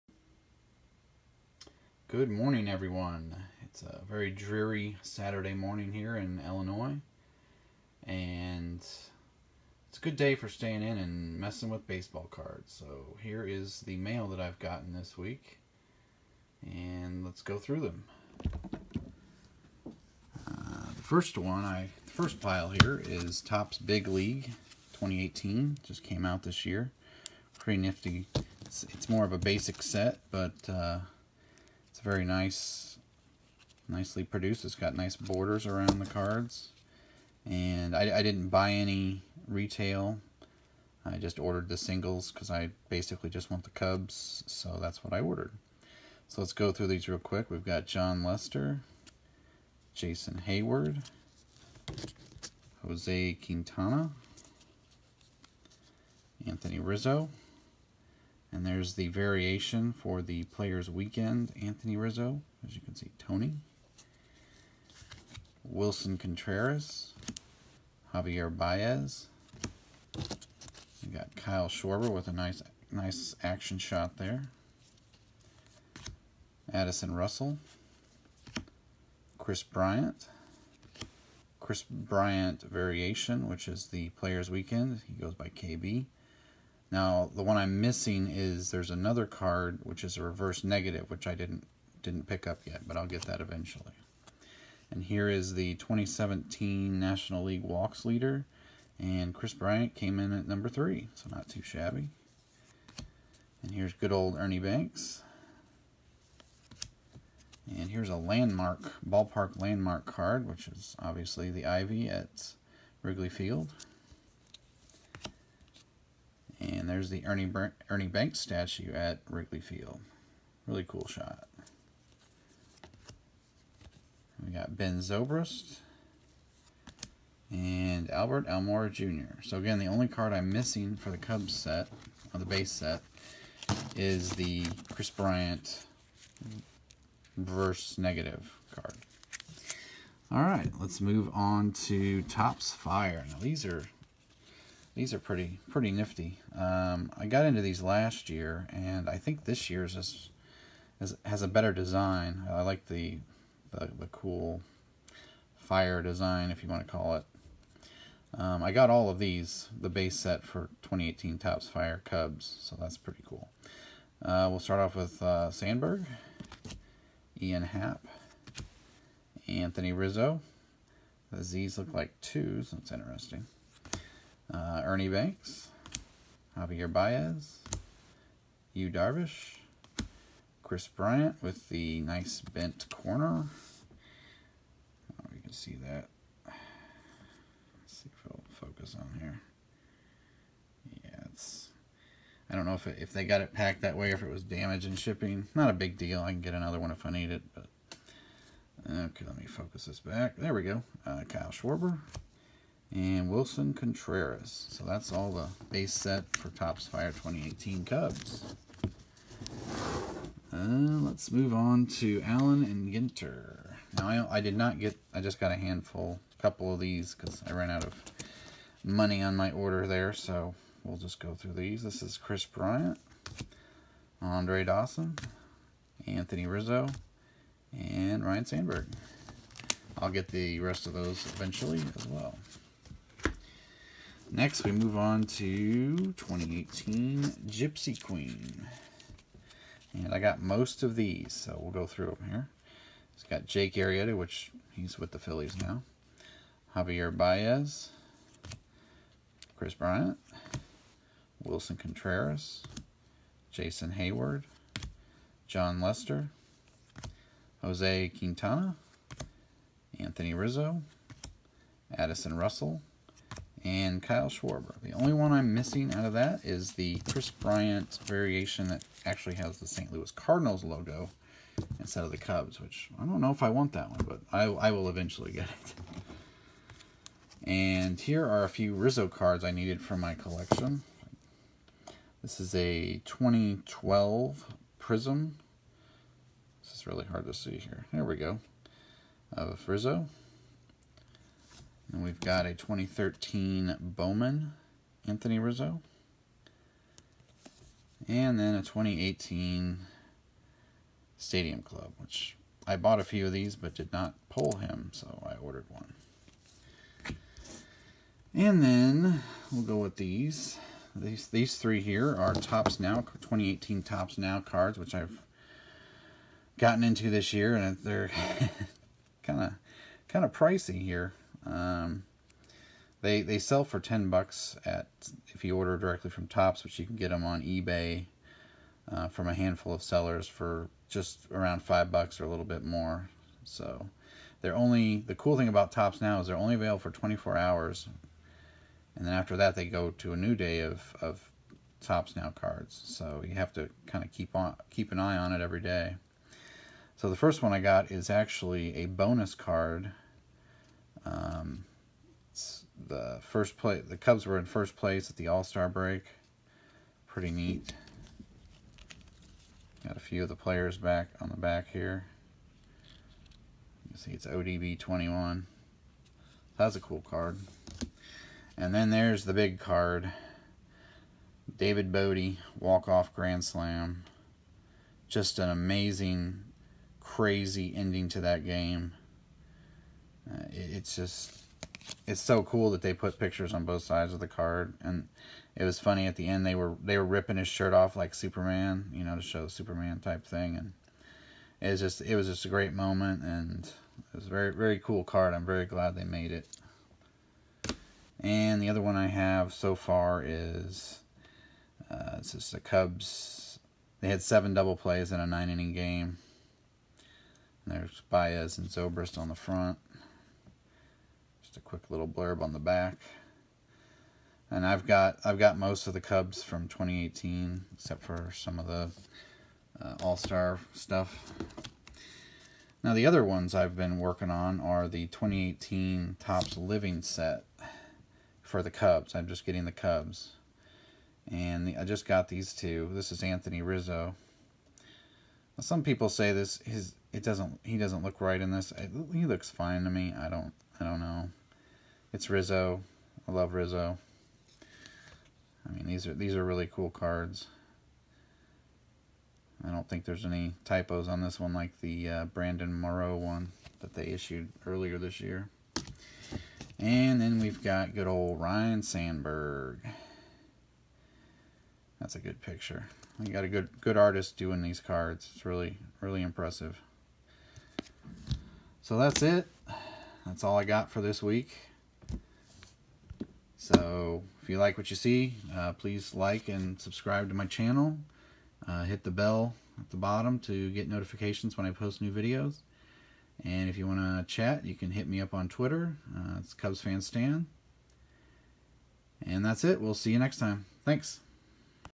CubsFanStan MailDay - 09/08/2018
I ordered a bunch of base cards mainly from Sportlots to fill in some Cubs team set needs. I also picked up some Anthony Rizzo cards for my PC.

2012 Panini Prizm #28 Anthony Rizzo
2013 Bowman Base Set #177 Anthony Rizzo
2018 Stadium Club Base Set #165 Anthony Rizzo
2018 Topps Allen & Ginter #5 Kris Bryant
2018 Topps Allen & Ginter #25 Andre Dawson
2018 Topps Allen & Ginter #32 Anthony Rizzo
2018 Topps Allen & Ginter #213 Ryne Sandberg
2018 Topps Big League #4 Jon Lester
2018 Topps Big League #52 Jason Heyward
2018 Topps Big League #73 Jose Quintana
2018 Topps Big League #91 Anthony Rizzo
2018 Topps Big League #91 Anthony Rizzo VAR Players Weekend
2018 Topps Big League #127 Willson Contreras
2018 Topps Big League #192 Javier Baez
2018 Topps Big League #197 Kyle Schwarber
2018 Topps Big League #202 Addison Russell
2018 Topps Big League #300 Kris Bryant
2018 Topps Big League #300 Kris Bryant VAR Players Weekend]
2018 Topps Big League #311 Joey Votto | Kris Bryant | Matt Carpenter
2018 Topps Big League #334 Ernie Banks
2018 Topps Big League #351 Bricks Ivy
2018 Topps Big League #357 Ernie Banks Statue
2018 Topps Big League #388 Ben Zobrist
2018 Topps Big League #393 Albert Almora Jr.
2018 Topps Fire #10 Ryne Sandberg
2018 Topps Fire #39 Ian Happ
2018 Topps Fire #72 Anthony Rizzo
2018 Topps Fire #73 Ernie Banks
2018 Topps Fire #74 Javier Baez
2018 Topps Fire #87 Yu Darvish
2018 Topps Fire #97 Kris Bryant
2018 Topps Fire #165 Kyle Schwarber
2018 Topps Fire #197 Willson Contreras
2018 Topps Gypsy Queen #70 Jose Quintana
2018 Topps Gypsy Queen #87 Anthony Rizzo
2018 Topps Gypsy Queen #95 Jon Lester
2018 Topps Gypsy Queen #115 Willson Contreras
2018 Topps Gypsy Queen #132 Kyle Schwarber
2018 Topps Gypsy Queen #135 Addison Russell
2018 Topps Gypsy Queen #147 Jason Heyward
2018 Topps Gypsy Queen #260 Kris Bryant
2018 Topps Gypsy Queen #278 Javier Baez
2018 Topps Gypsy Queen #290 Jake Arrieta
2018 Topps Living Set #64 Ryne Sandberg
2018 Topps Living Set #67 Anthony Rizzo
2018 Topps Now #591 David Bote RC
2018 Topps Now #609 Chicago Cubs
2018 Topps Now Road to Opening Day Bonus ODB-21 Chicago Cubs